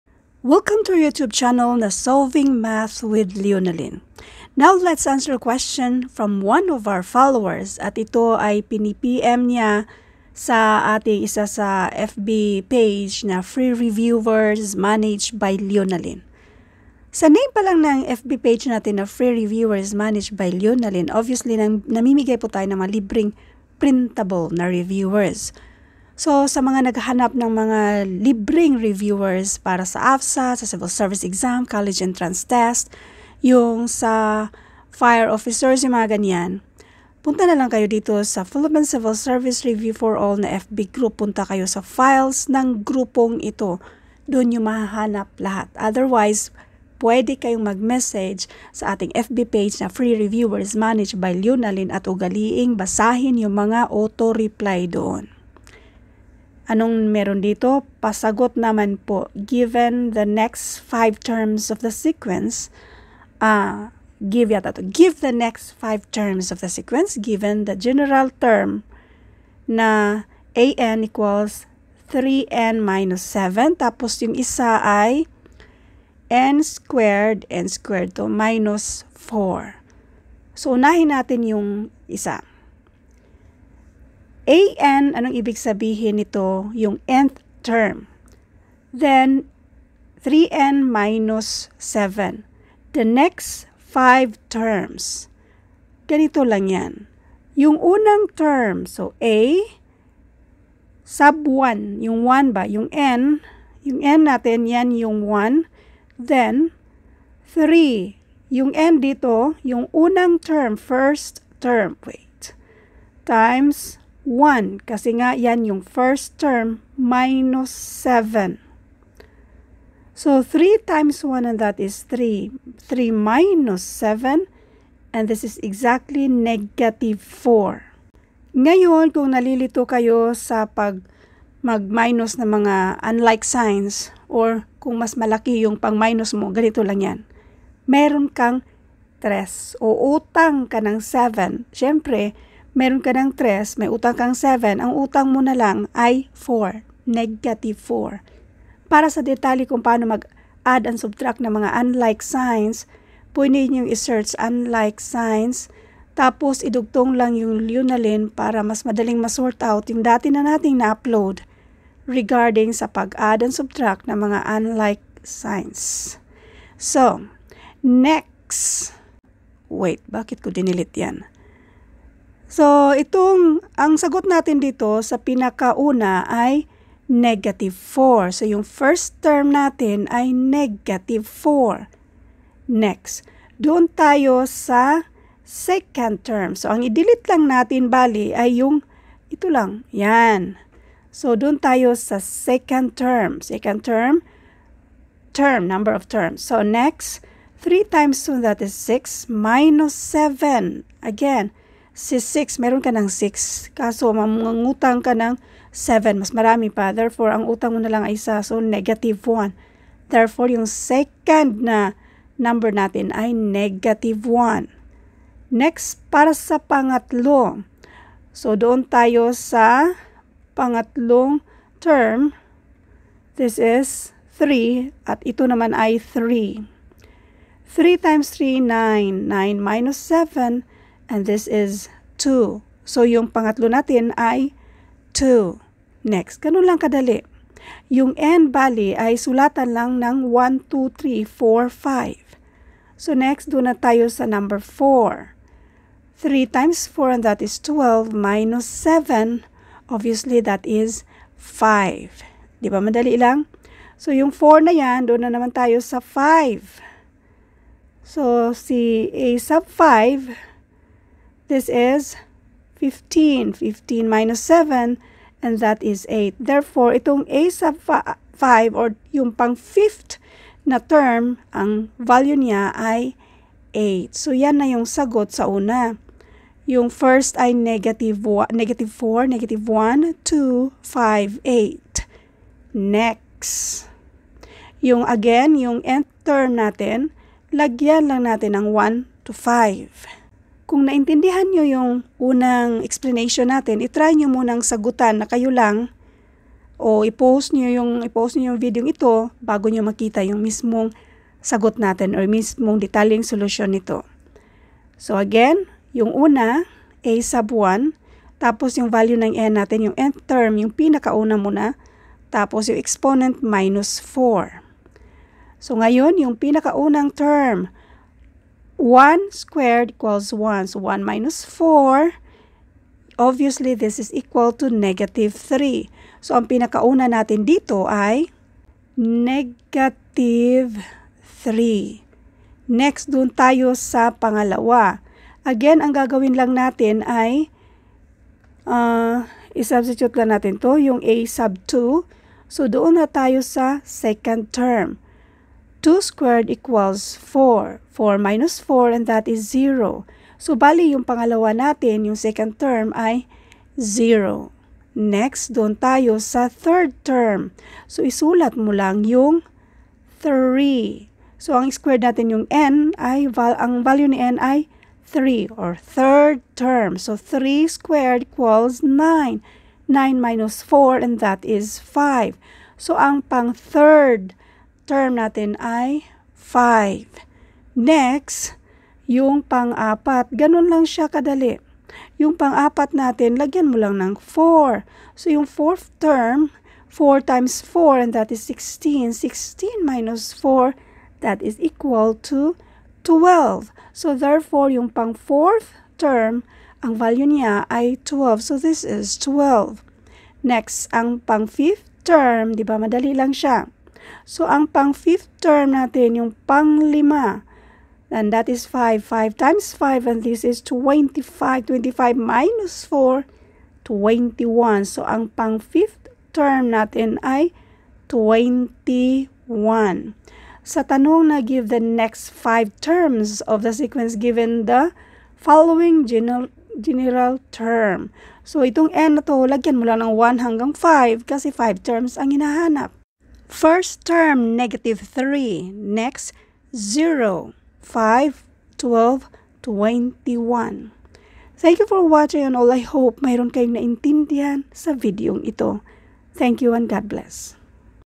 Give the next 5 terms of the sequence given the following general term an = 3n – 7 and an = n² – 4
Give the next 5 terms of the sequence given the following general term
an = 3n – 7
an = n² – 4

Free Civil Service Reviewers managed by Leonalyn

Philippine Civil Service Review for all

Editing Apps: VivaVideo
SamsungNote5: Snote
Back-up: SamsungNote9